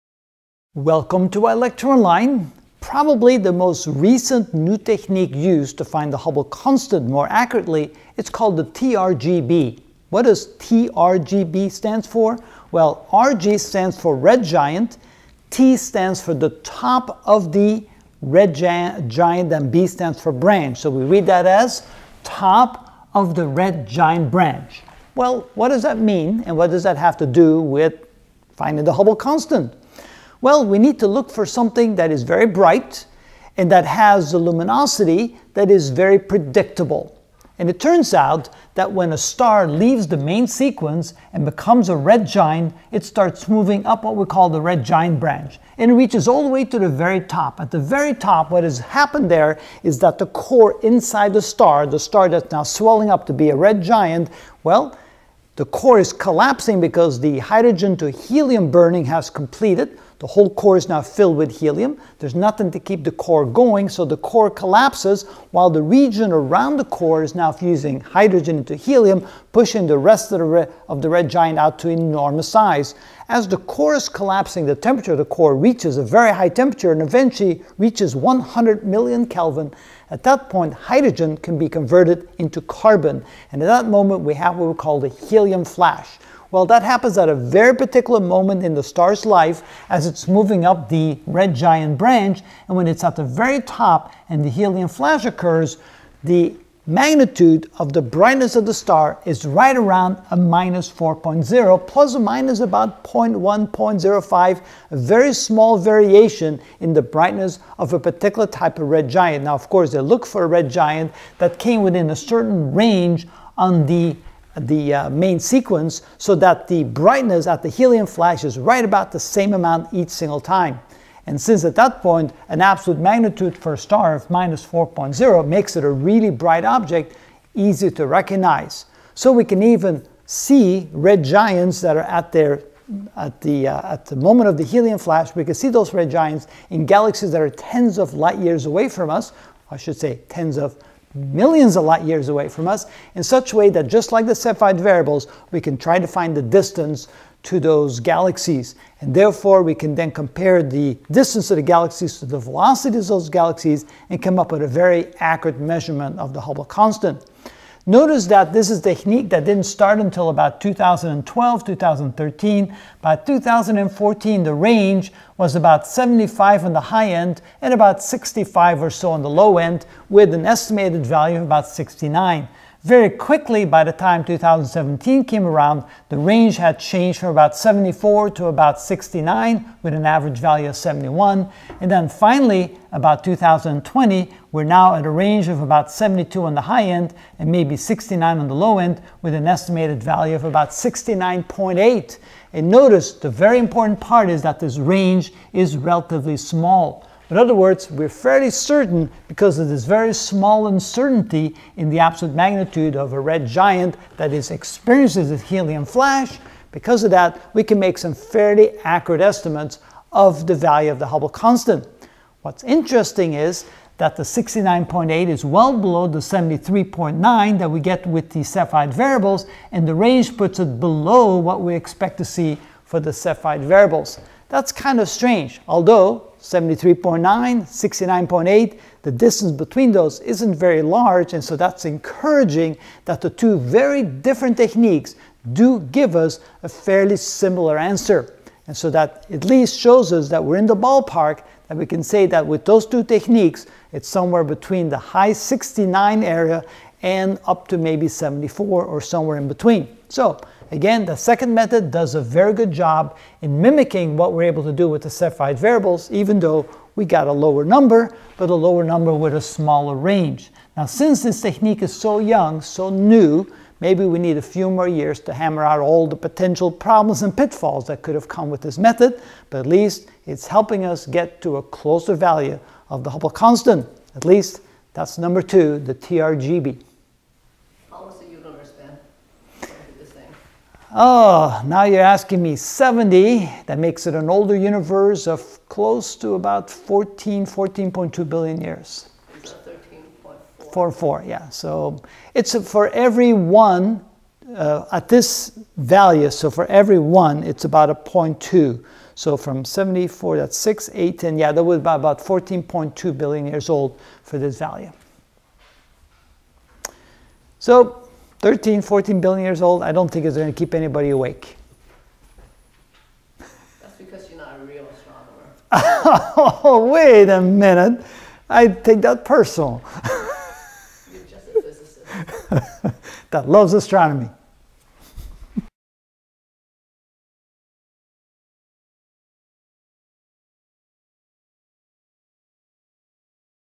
Astronomy - Ch. 26: Hubble Law (12 of 20) The Latest 4 Key Technique 2) TRGB
We will learn the 2nd of the 4 latest techniques (not using Cepheid variables) of calculating the Hubble Constant: 2) TRGB (Top if the Red Giant Branch).